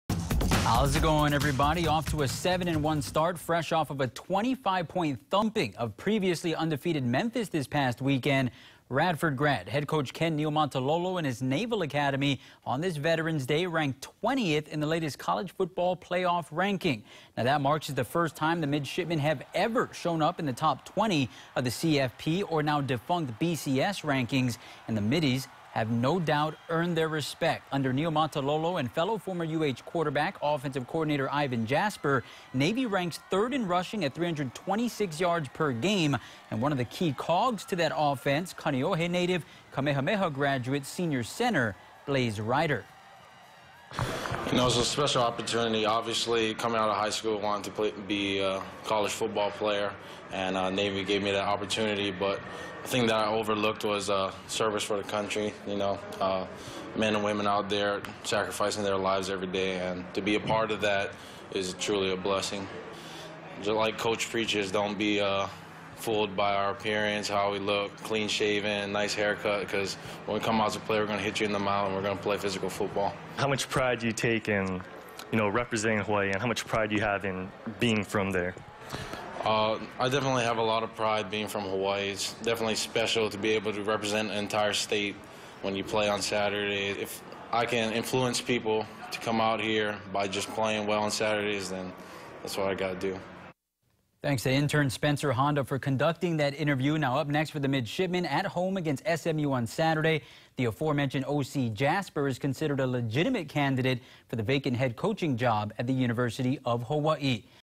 Navy center Blaze Ryder proud to represent Academy, Hawaii
One of the key cogs to the Midshipmen offense is Kaneohe native and Kamehameha graduate, senior center Blaze Ryder.